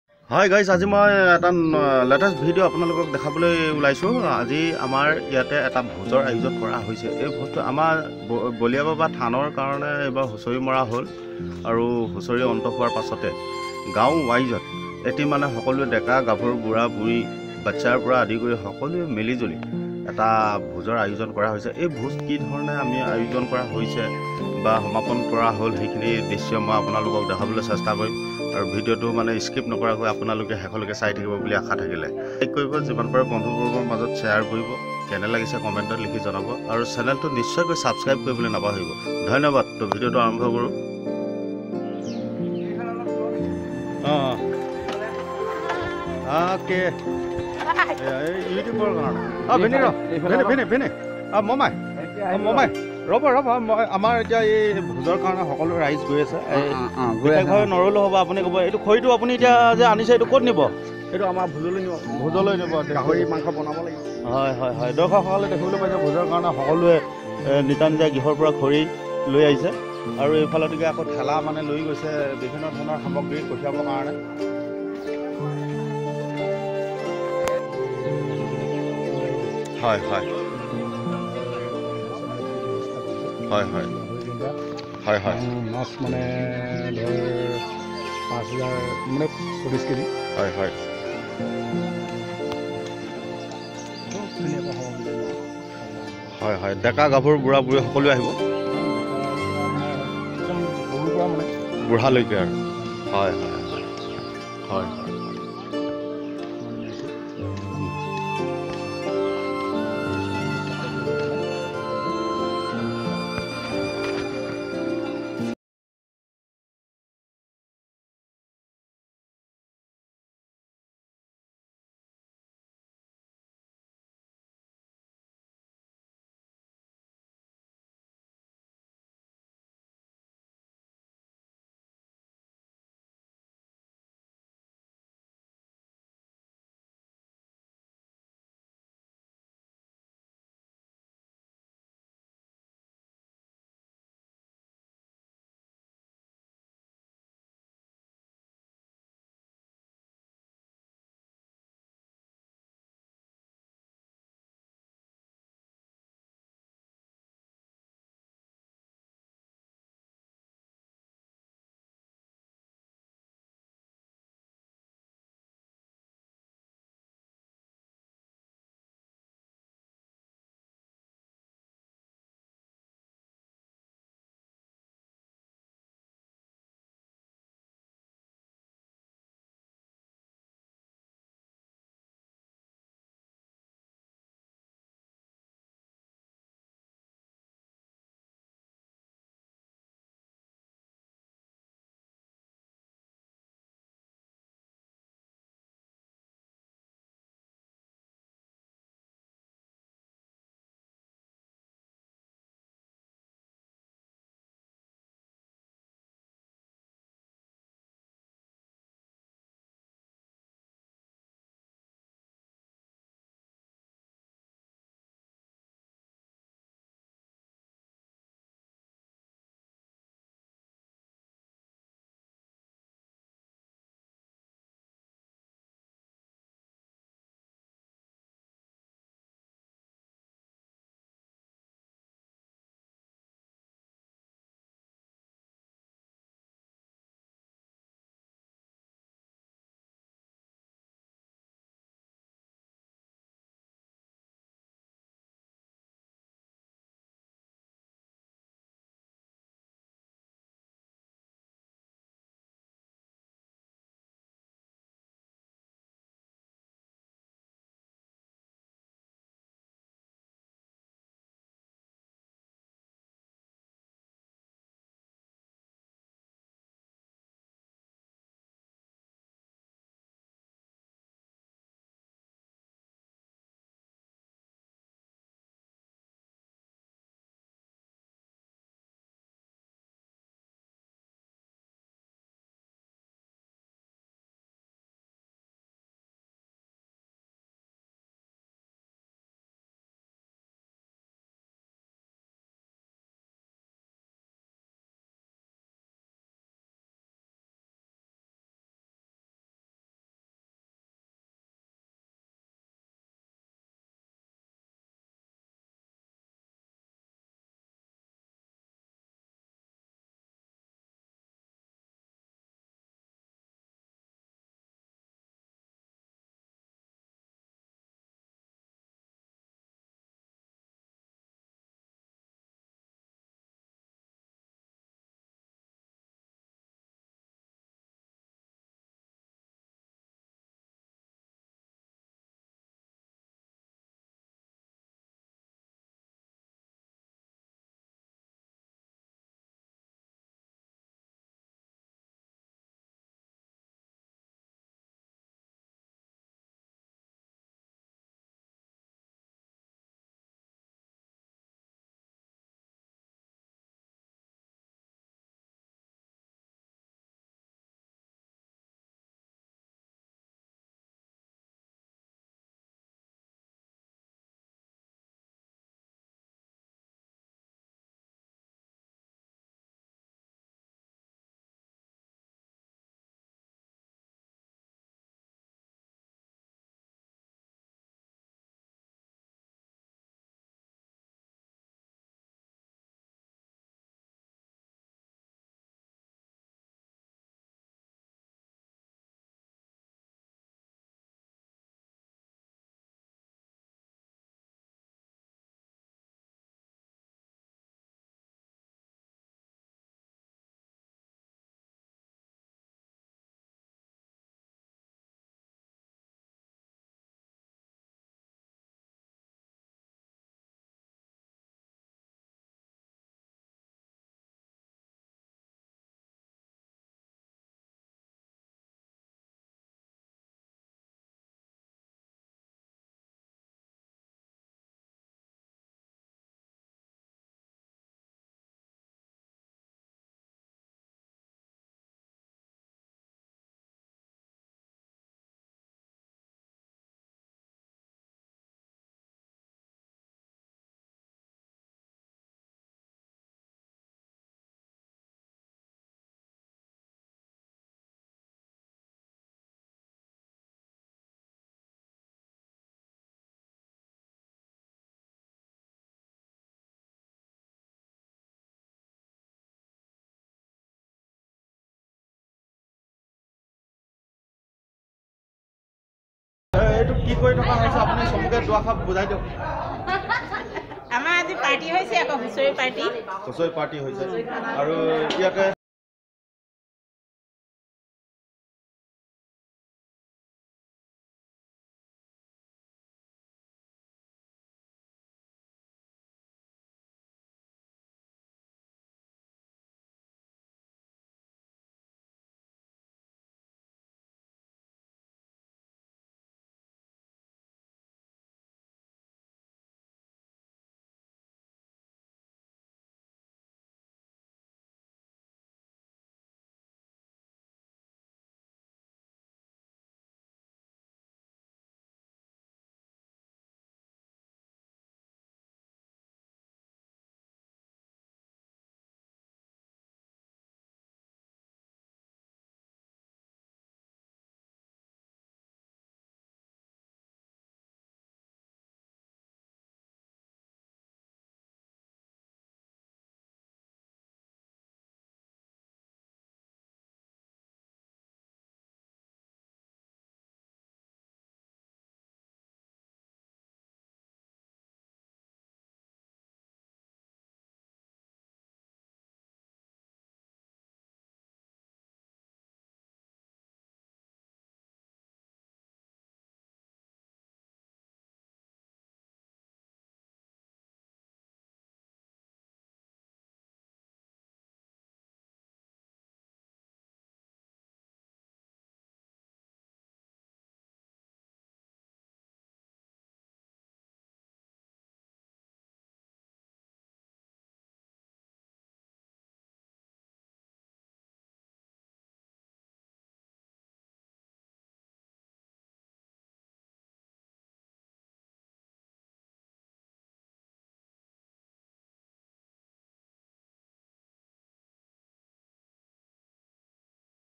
দেউৰী সকলৰ এটা উল্লেখনীয় ভোজৰ অনুষ্ঠান //part 1
hello friends
i m prashanta deori, today I bring one letest VEDIO about deori social bhujan after husari,singioni villagewise .